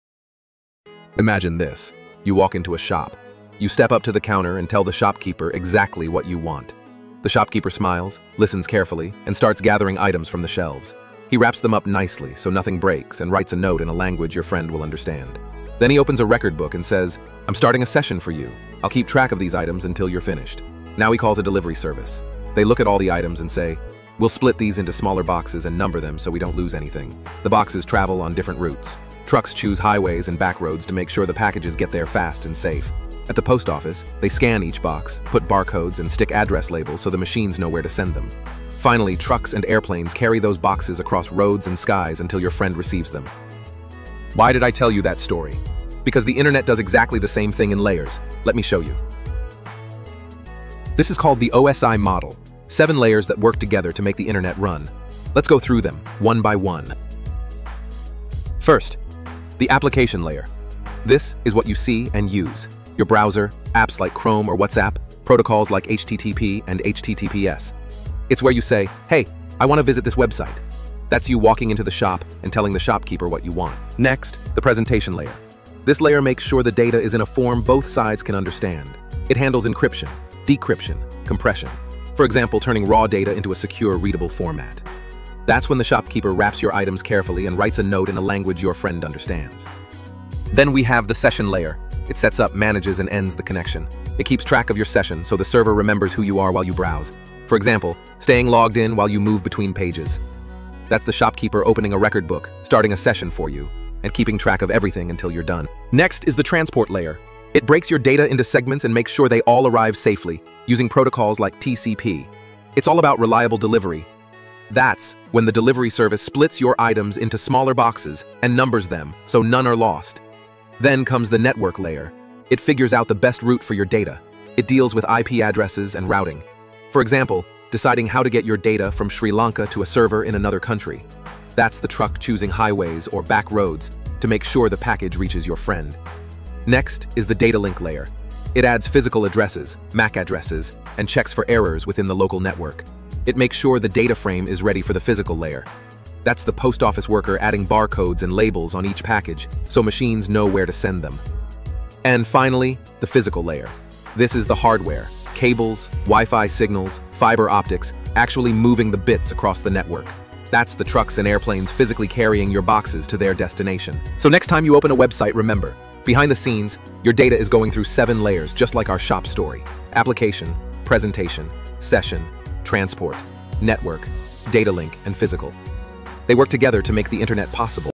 OSI Model Explained with a Shop Story (Easiest Way Ever)
Ever wondered how your data travels across the internet? In this video, I explain the OSI model using a fun shop story analogy. We’ll go through all 7 layers — Application, Presentation, Session, Transport, Network, Data Link, and Physical — and connect each layer to a real‑world example.

By the end of this video, you’ll not only understand the OSI model, but you’ll never look at browsing the same way again!

💡 Topics covered:
✔️ What each OSI layer does
✔️ Real‑world shop analogy for each layer
✔️ Visual guide to remember all 7 layers